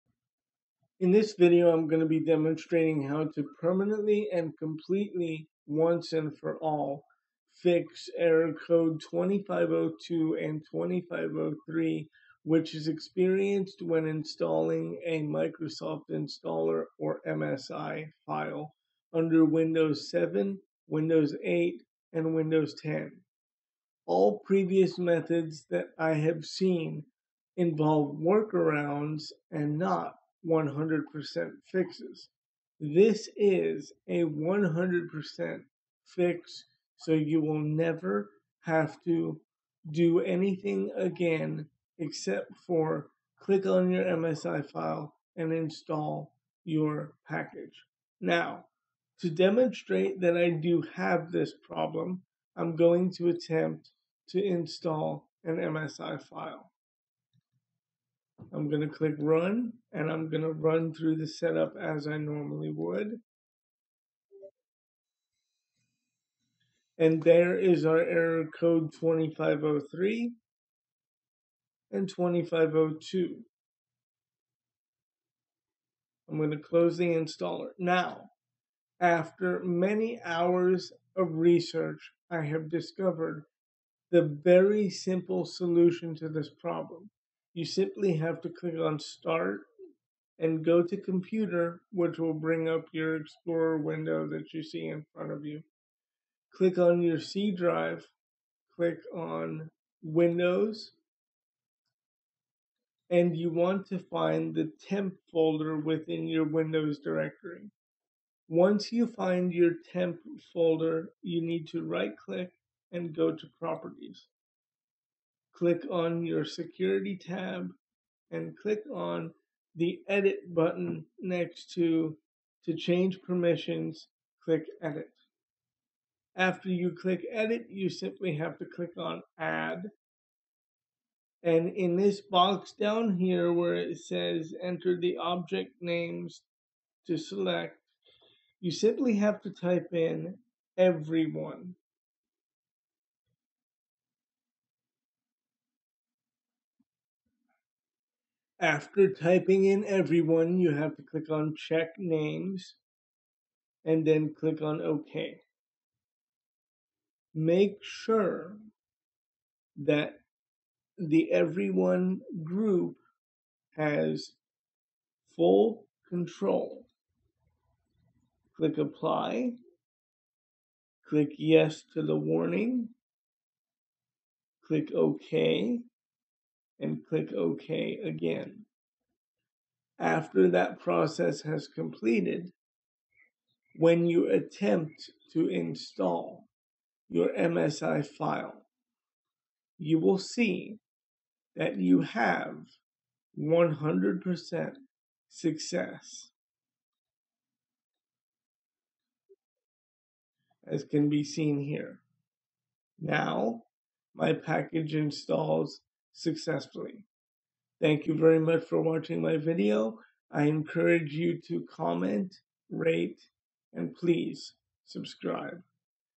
Complete Permanent Fix for Windows Installer Error 2502 & 2503 Windows 7/8/8.1/10 NOT A WORKAROUND
In this video I demonstrate what people have been searching for for a long time, they complete and permanent solution to Windows installer error code 2502 and 2503